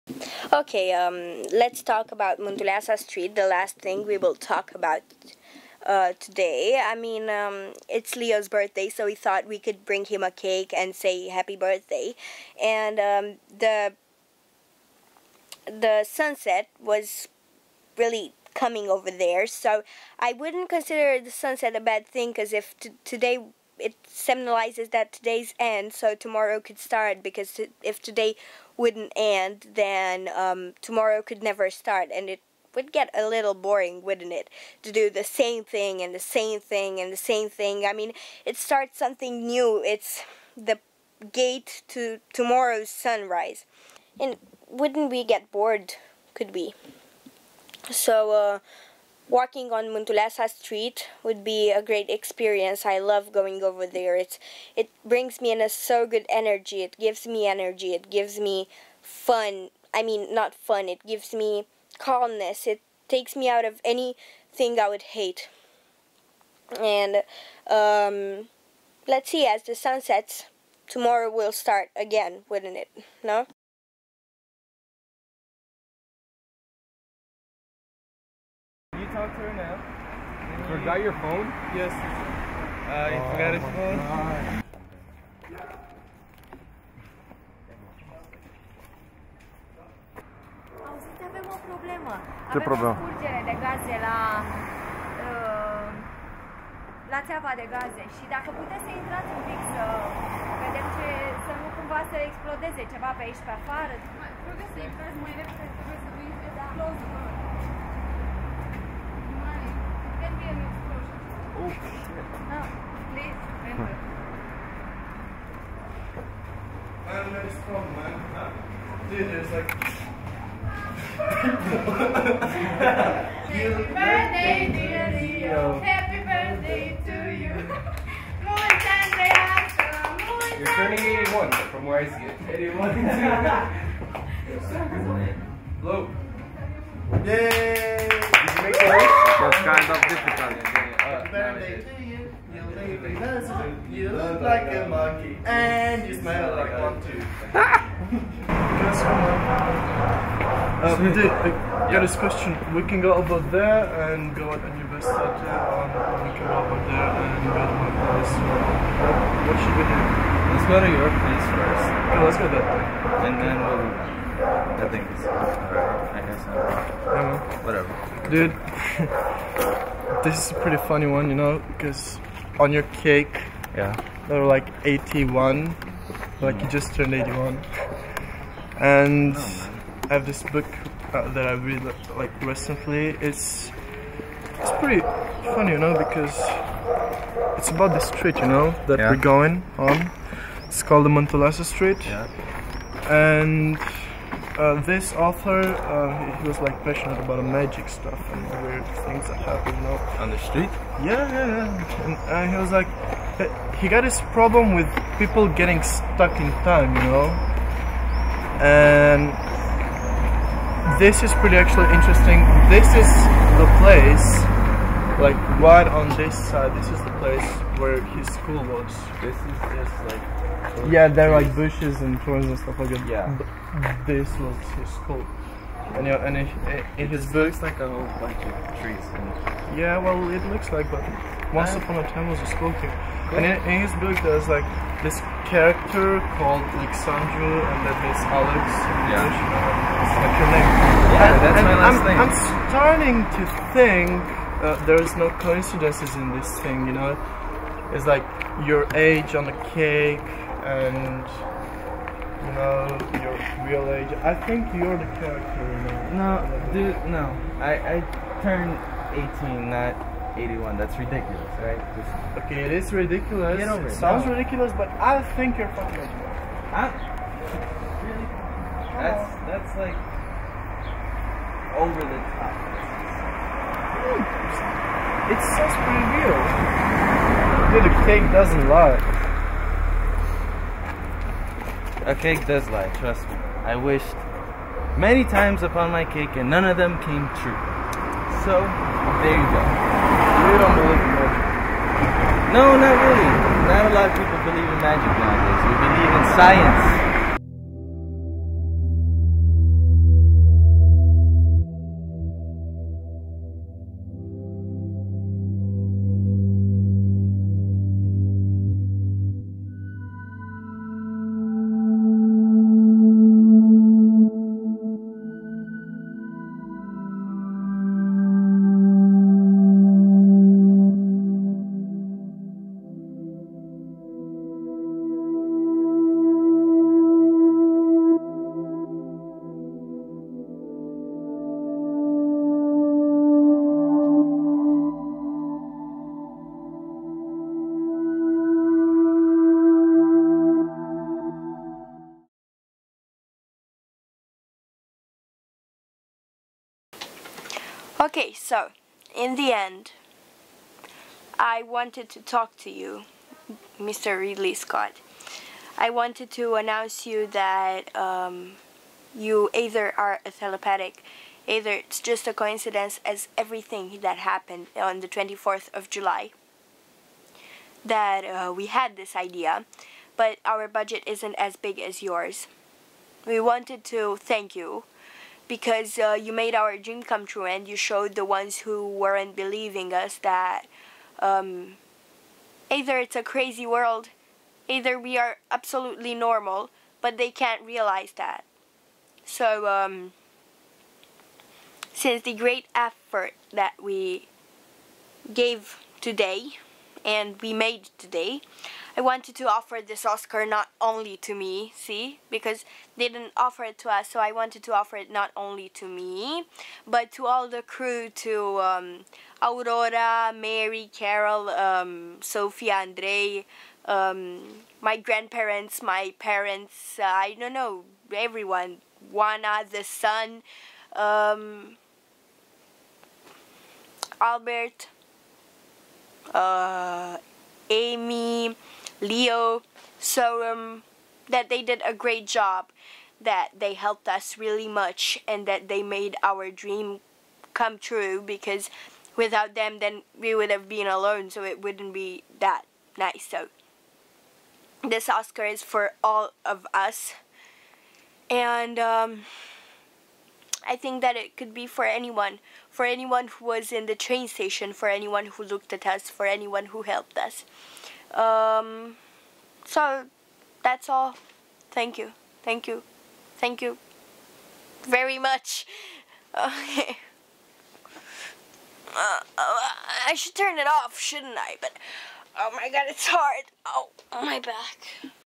12684-4
20:00 We had Leo believed the building where he was is going to explode meanwhile we made him a surprise and took him a cake. July 24th  was also his birthday. Albert had the idea to make this surprise and organized everything.
We also think  that Leo is 18y old and 81y old undercover. That's because we are on a magic street where magical things happen. To find more you must read Mircea Eliade's book - On Mantuleasa Street ( english title The Old Man and the Bureaucrats ).
The sunset comes on Mantuleasa street , waiting for another sunrise.
And a big THANK YOU in THE END.

Camera: Andrei Baltaretu
Editing: Edward Gunther Popovici